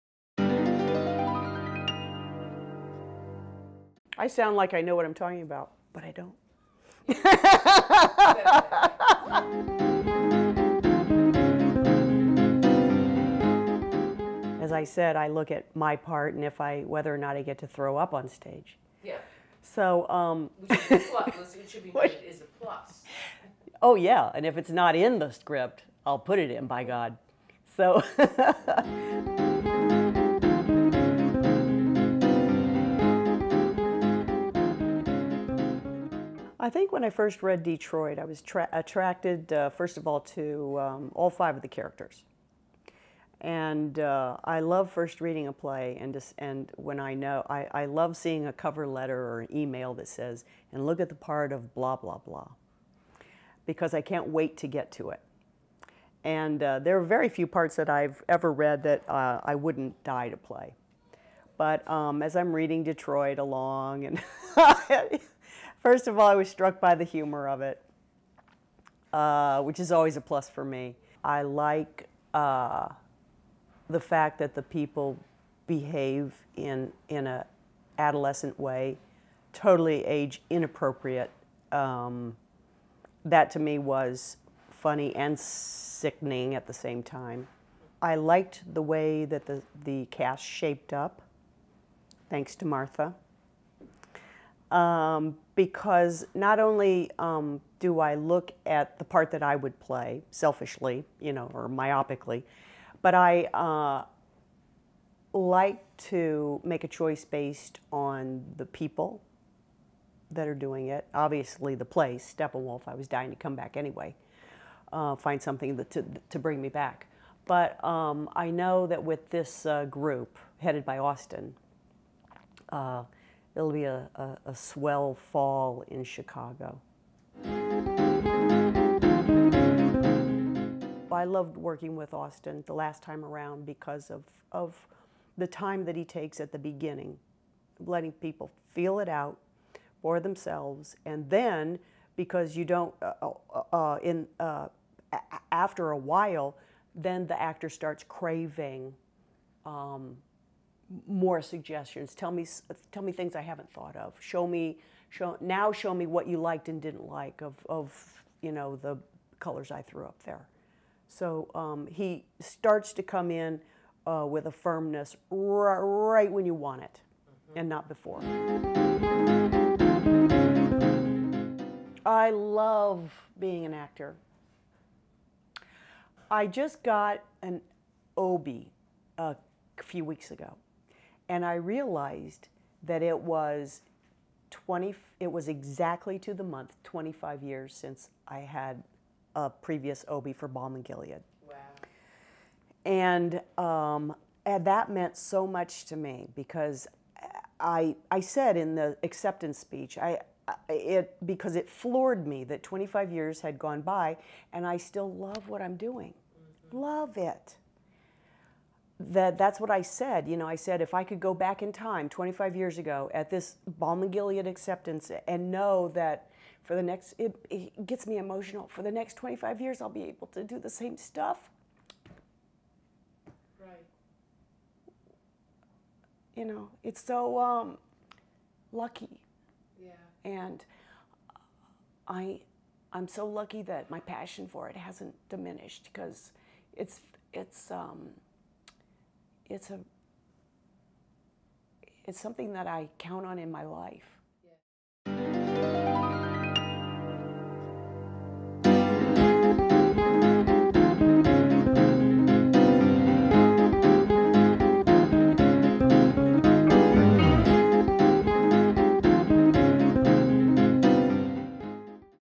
Getting to Know Ensemble Member Laurie Metcalf
Ensemble member Laurie Metcalf talks about Detroit, the first show in the 2010-11 season, working with director and ensemble member Austin Pendleton, and the role of acting in her life.